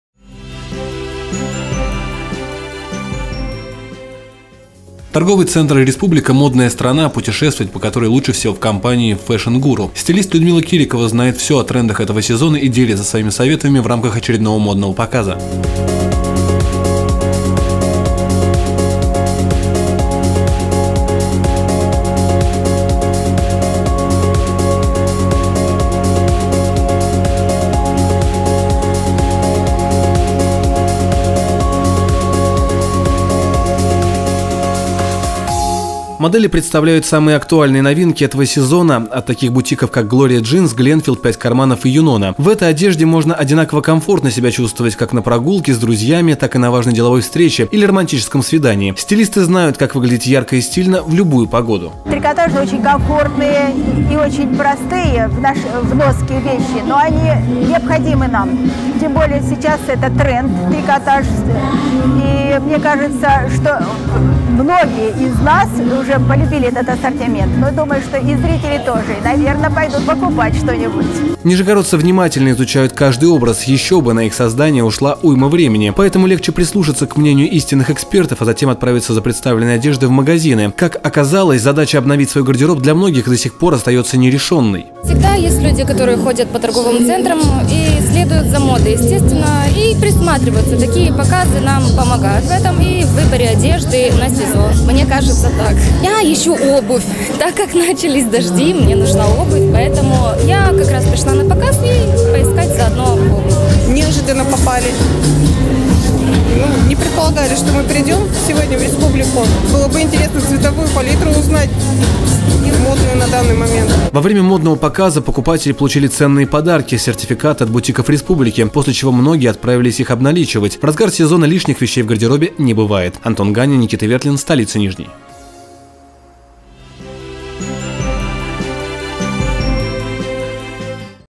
Показ модной одежды прошел в ТЦ «Республика» (0+)
© Медиапроект «Столица Нижний»
Показ модной одежды прошел в ТЦ «Республика» 5 октября 2019 года. Профессиональные стилисты познакомили гостей с новыми коллекциями одежды сезона осень—зима 2019/2020 и помогли грамотно составить модные образы. В рамках традиционного дефиле на подиуме были представлены актуальные новинки сезона от бутиков Gloria Jeans, Glenfield, «5Карманов» и «Юнона». Во время модного показа покупатели приняли участие в розыгрыше ценных подарков — сертификатов от модных бутиков «Республики».